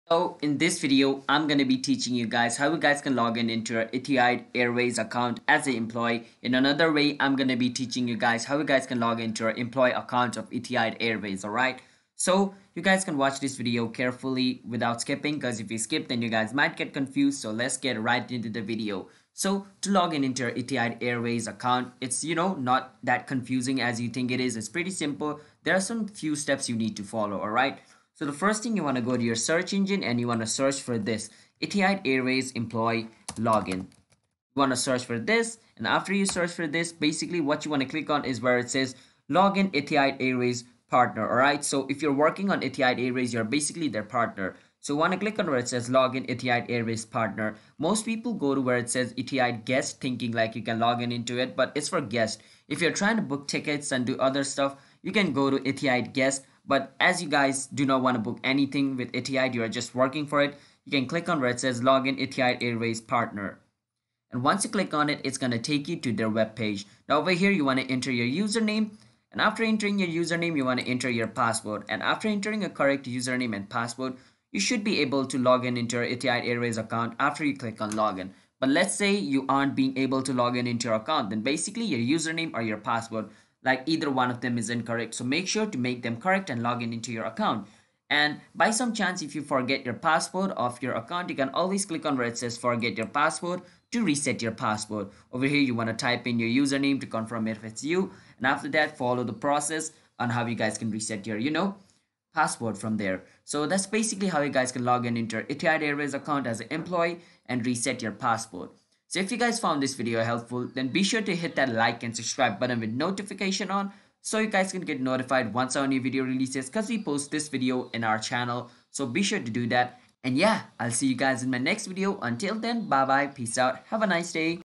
Etihad Airways Employee Login | Login to Etihad Airways Account (2021)
In this short and simple tutorial, I’ll be showing you how you can login to Etihad Airways employee account. This video will be helpful to anyone trying to log into their account easily and quickly.
Etihad Airways is the second flag carrier of the United Arab Emirates. Its head office is in Khalifa City, Abu Dhabi, near Abu Dhabi International Airport. Etihad commenced operations in November 2003. It is the second-largest airline in the UAE after Emirates.
So, to log into your Etihad Airways employee account, follow the steps mentioned below:
1. The first thing you’ll need to do is open your browser and then search for ‘etihad airways employee login’
2. Click on the link which says Login Etihad Airways Partner, from the list of results, displayed and you’ll be forwarded to their website.
3. Then, you’ll have to enter the username and password assigned to your account.
4. Once you do that, click on Log In and you’ll be successfully logged into your account.
After following all the steps mentioned, you’ll be able to log into your Etihad Airways employee account.

Timestamps:
0:00 Intro
0:19 Etihad Airways employee log in
1:43 Outro